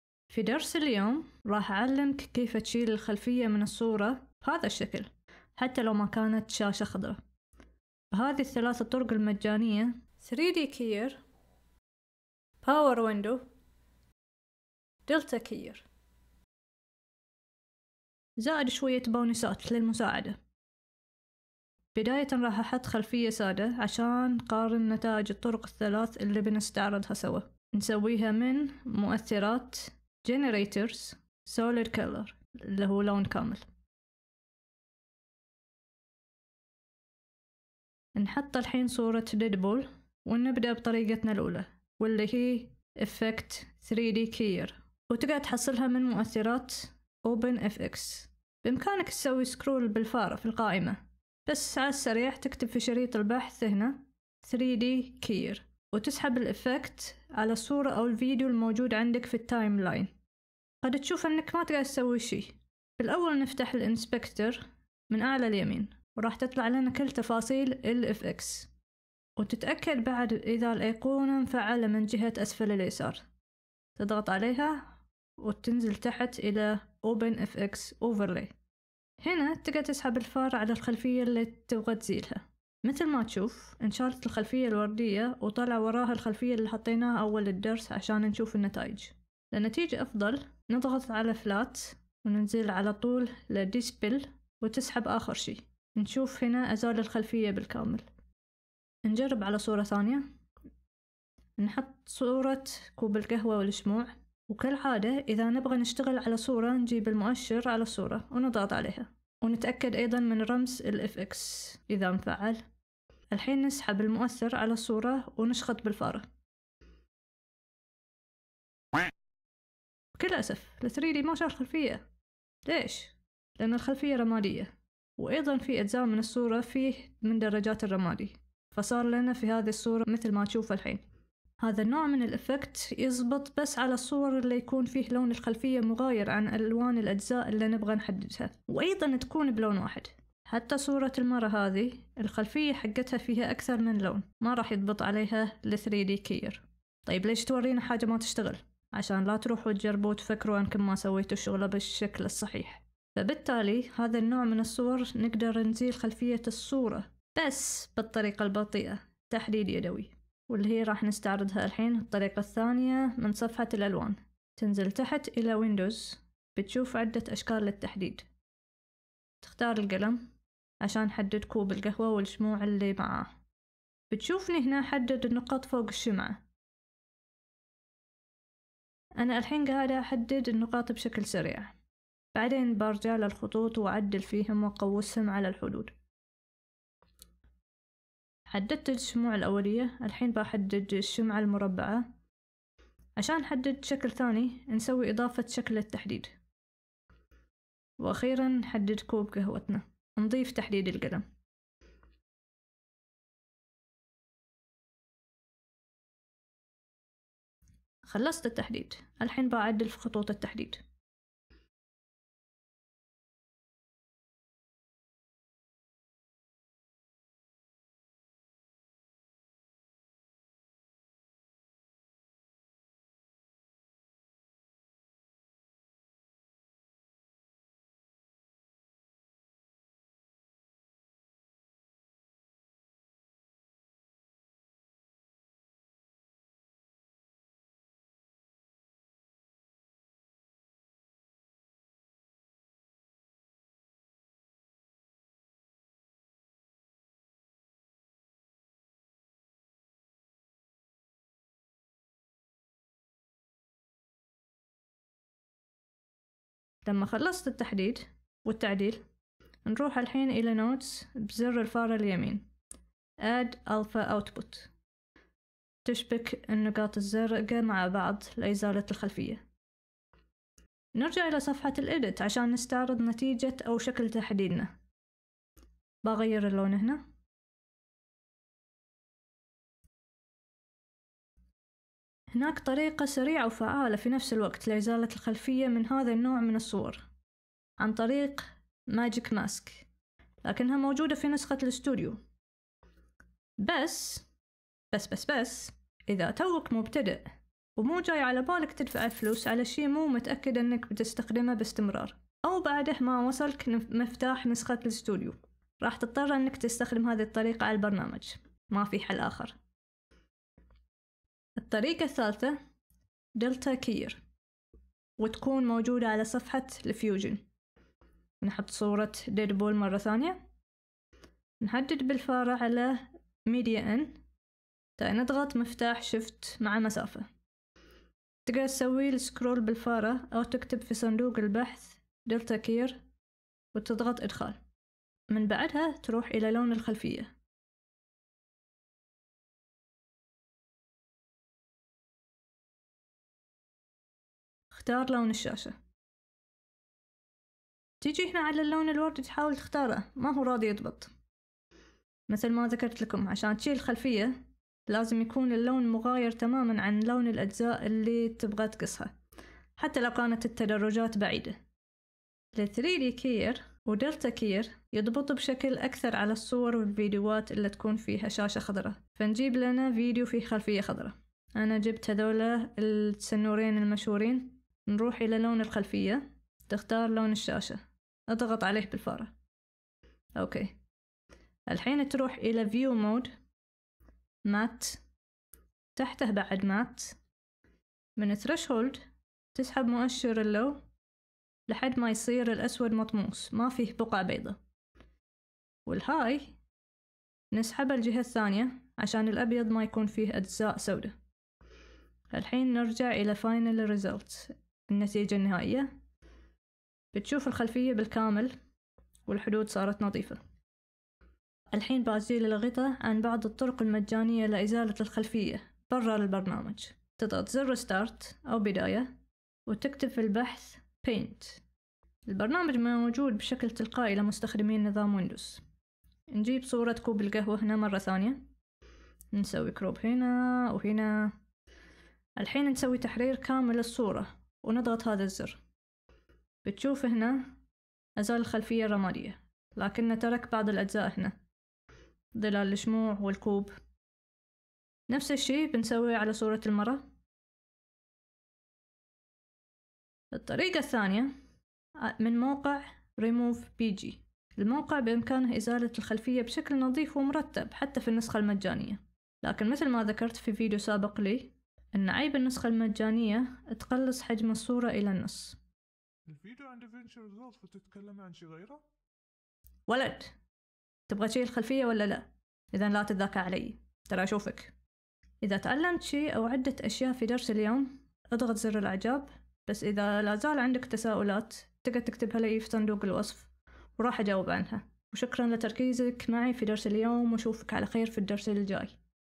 إزالة الخلفية من الصورة- دفنشي ريزولف
نستعرض 3 طرق لإزالة الخلفية على دفنشي ريزولف... مزاياها وعيوبها
والتي هي
3D keyer  00:42
Power Window 2:33
Delta Keyer  5:12